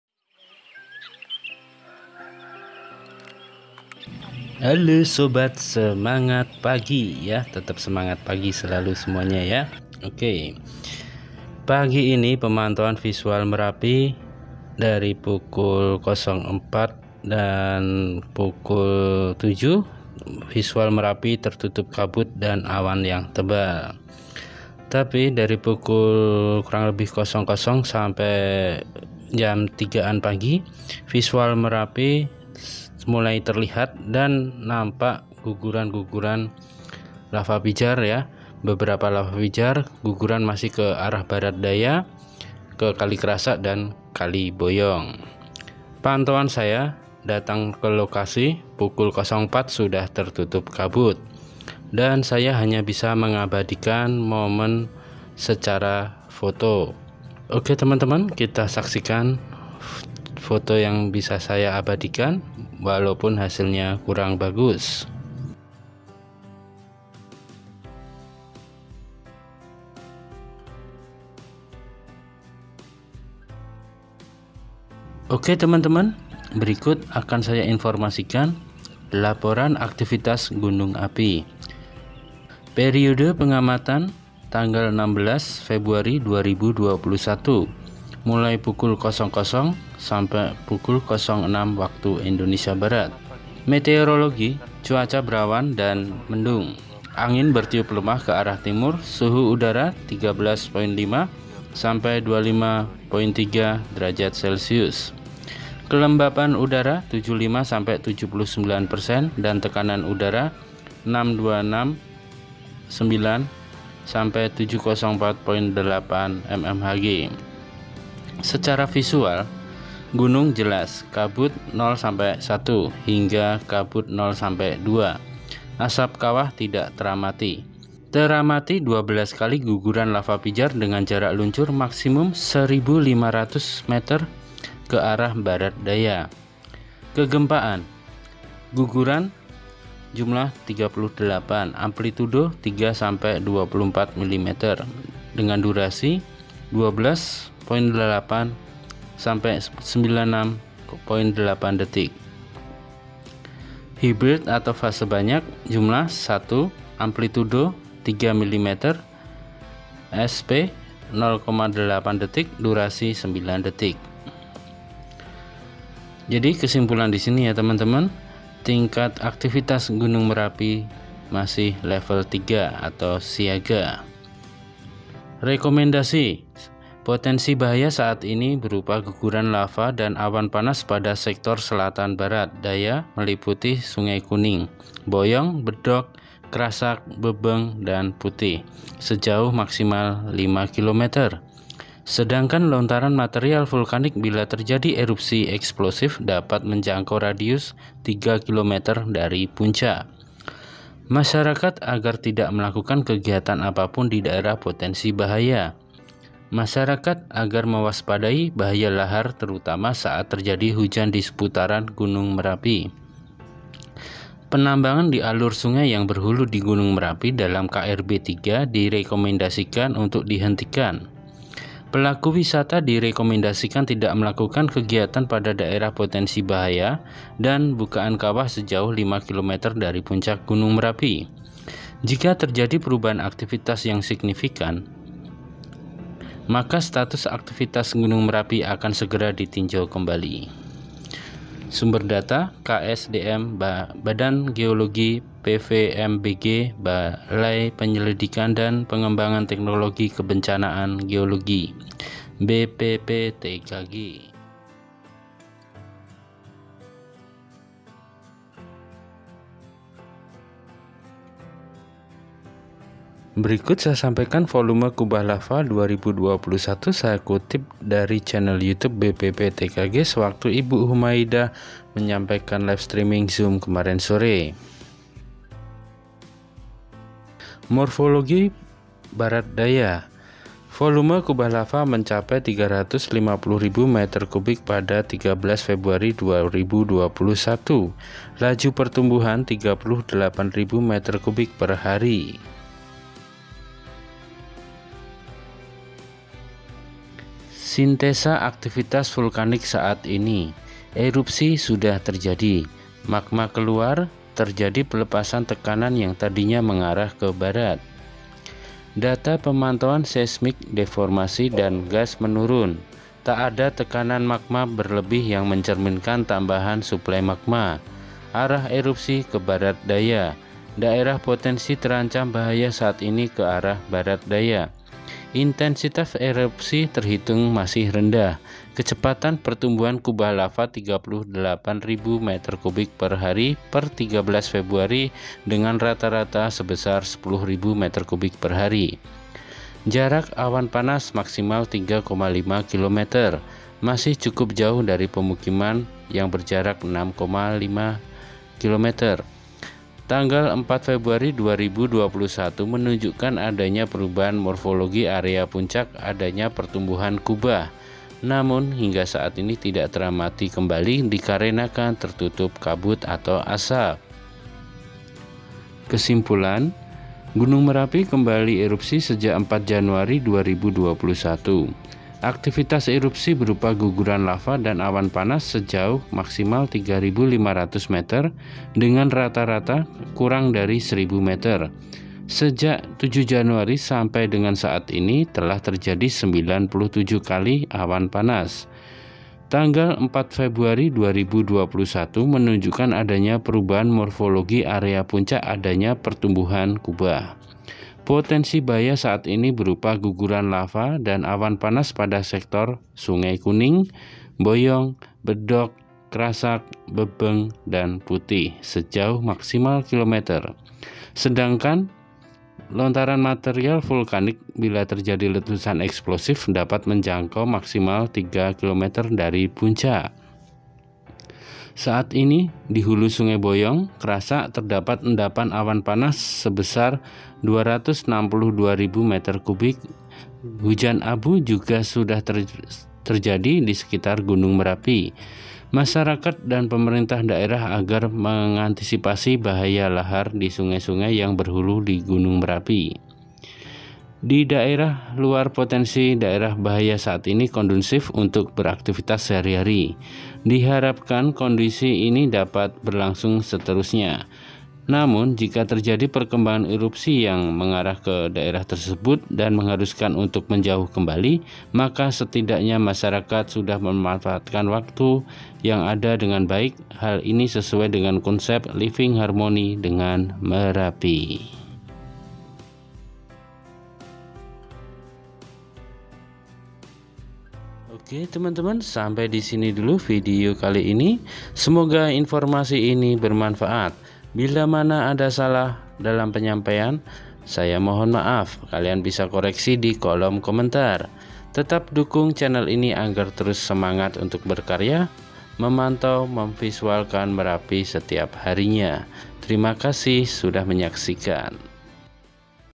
Tambah Besar Volume Kubah Lava 2021
Info Gunung Merapi Terkini Volume Kubah Lava 2021 dan Pantauan visual Merapi hari ini 16  Februari 2021, masih terpantau beberapa guguran lava pijar Merapi ke arah Barat daya atau kali krasak dan sungai boyong